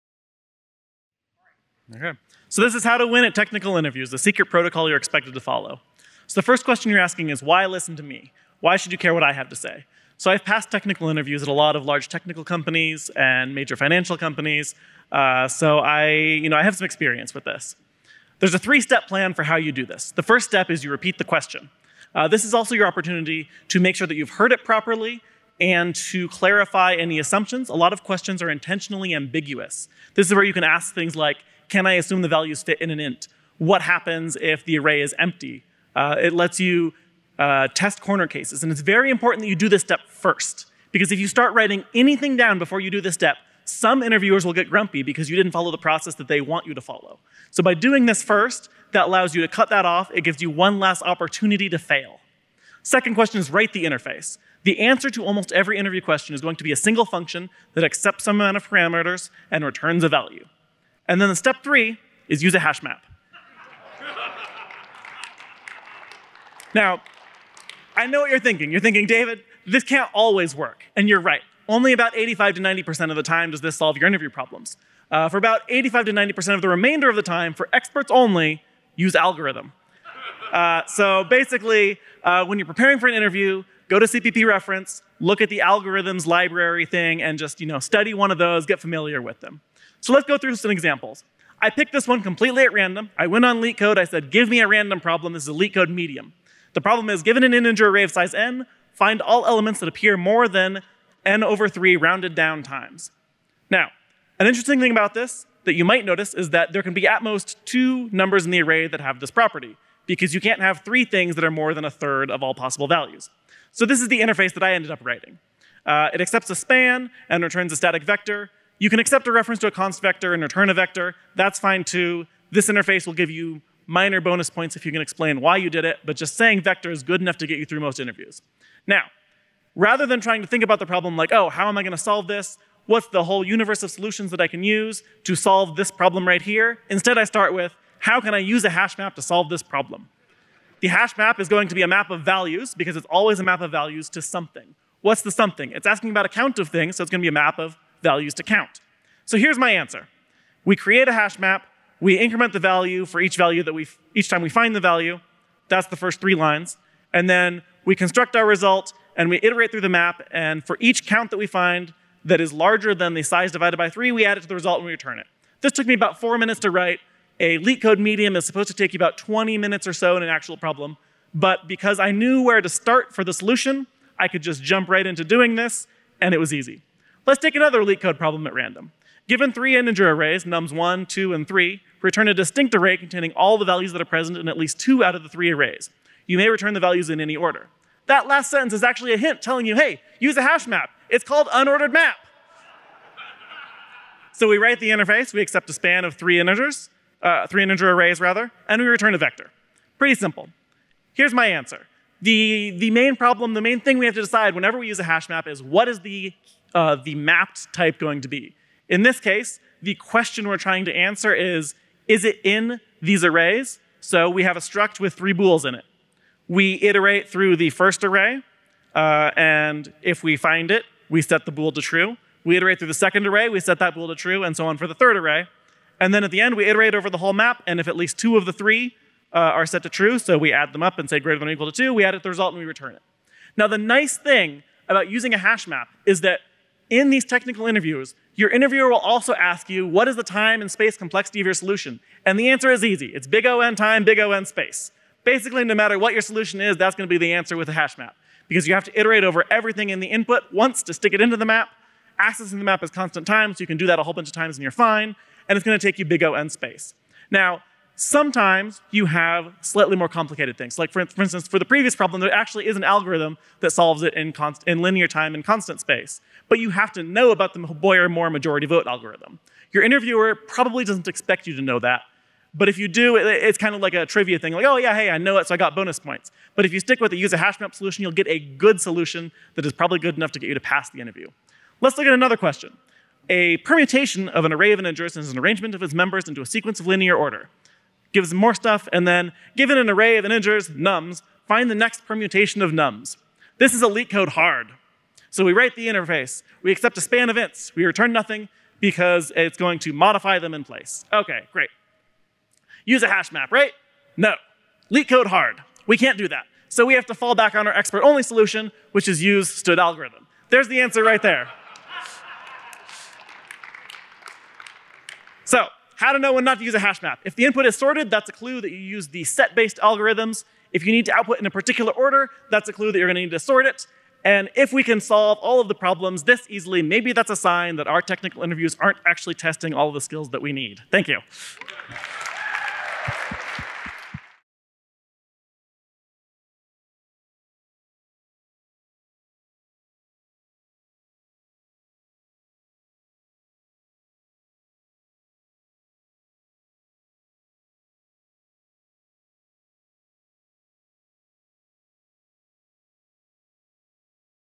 Lightning Talk: How to Win at Coding Interviews - David Stone - CppCon 2022
5 minutes of preparation is plenty to beat any coding interview.

David Stone

David Stone has worked on autonomous vehicles, large-scale distributed systems, and now works developing software for high-frequency trading. He is a member of the C++ Standardization Committee, where he chairs the Modules Study Group (SG2) and is the vice chair of the Evolution Working Group (EWG).